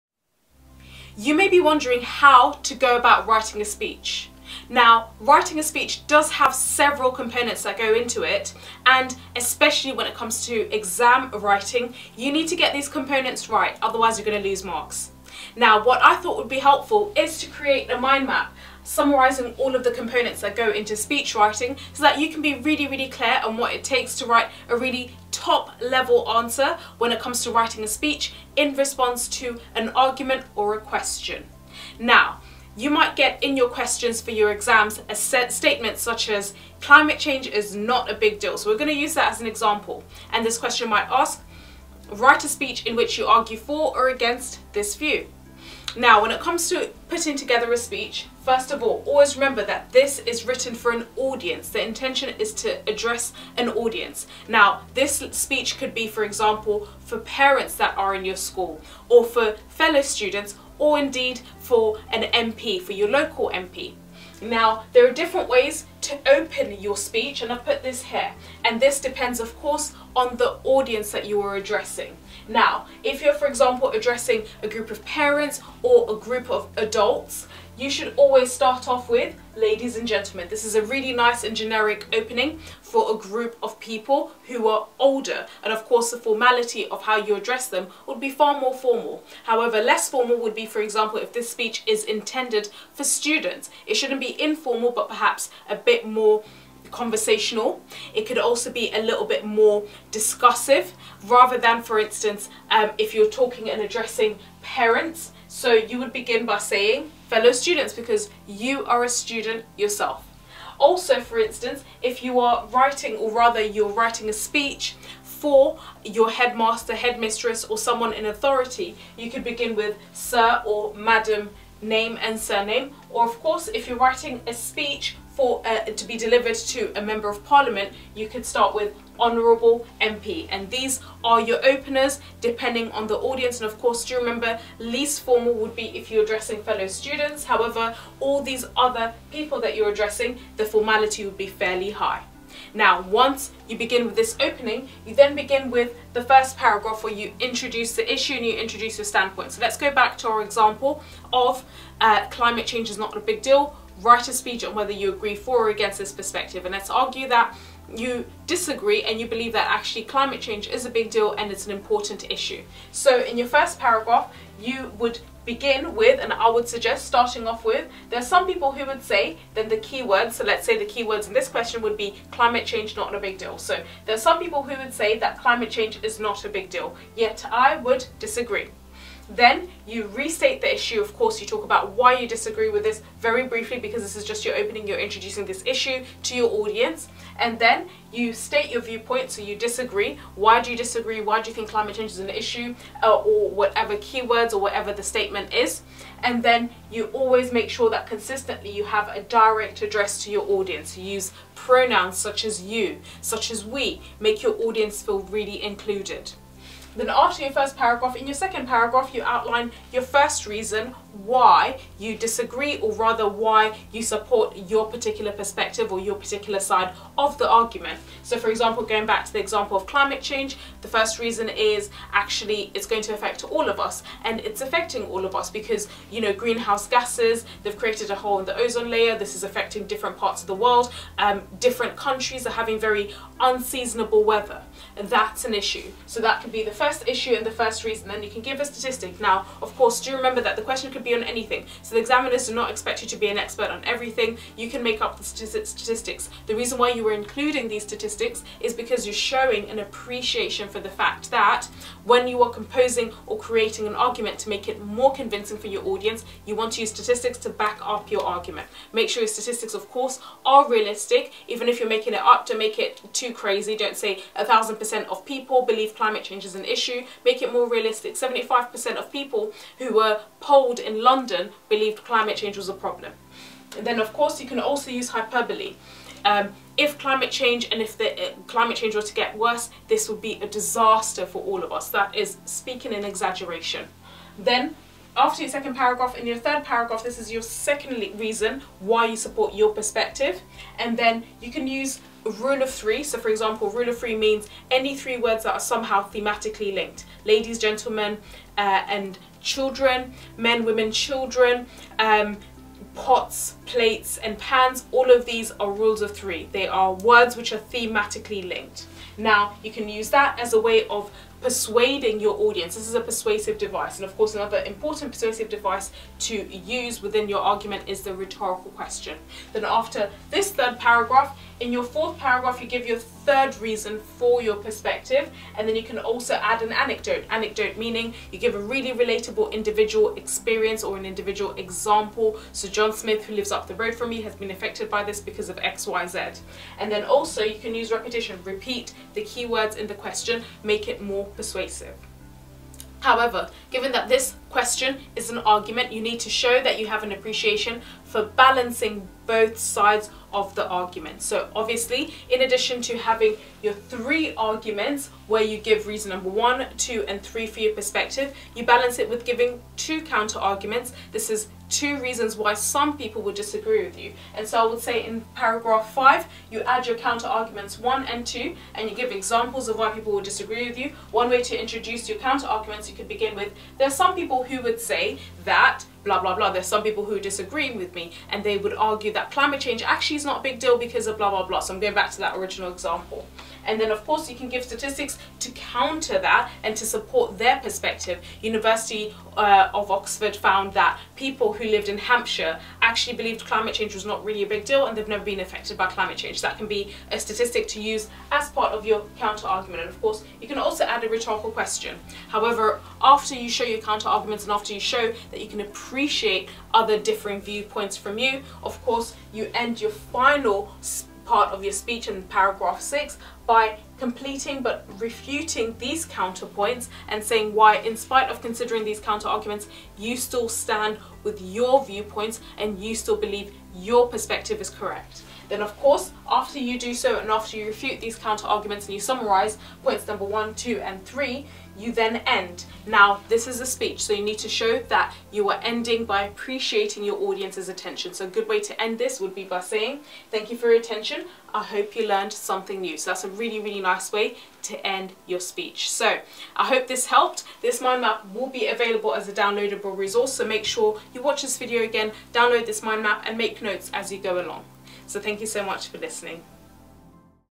English Language Paper 2 Question 5 Speech Model Answer: How To Achieve Grade 9 For AQA GCSE Exams!
In this video, we're going to answer English Language Paper 2 Question 5 Speech Model Answer: How To Achieve Level 9 For AQA GCSE Exams!

If you're looking to raise your English exam score, then be sure to watch this video! We'll show you how to achieve a 9 out of 10 on your English exams by using the speech model answer. This method is proven to help students achieve high marks on their English exams, so make sure to watch this video and learn all about it!

Barbara will show you an easy-to-follow mind map of how to tackle this questions and get top marks!